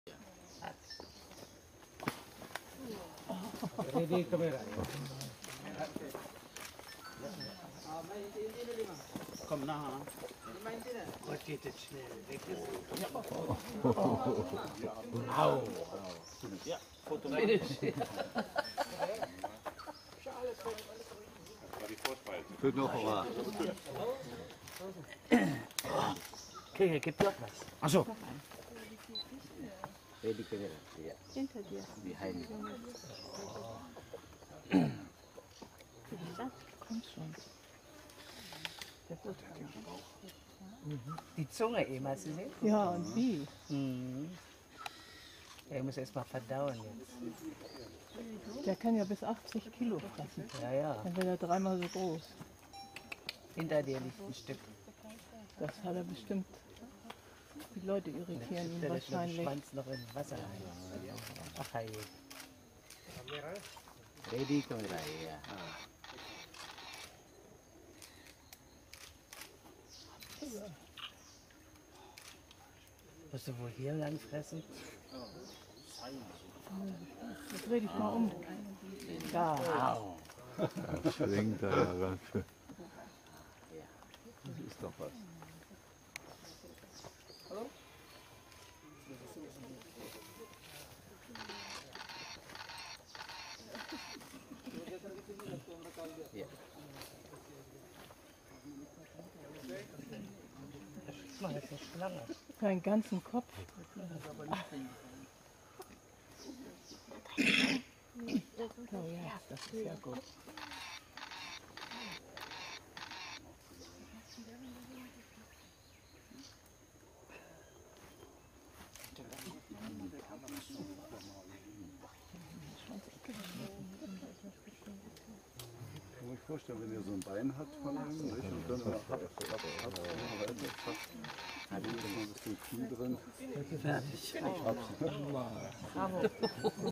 Komodowaran verspeist Ziege
Während der Führung über die Insel Komodo durften wir an der Fütterung einer der Riesenechsen teilhaben. Dabei wird dem Tier eine komplette Ziege in zwei Hälften zugeworfen. Laut unserem Guide werden die Warana lediglich einmal im Monat gefüttert. Anschließend brauchen Sie etwa 3-6 Tage zur Verdauung. Schlecht verdauliche Teile wie Hufe, Zähne, Haare, etc. werden vorher bereits ausgewürgt. Siehe auch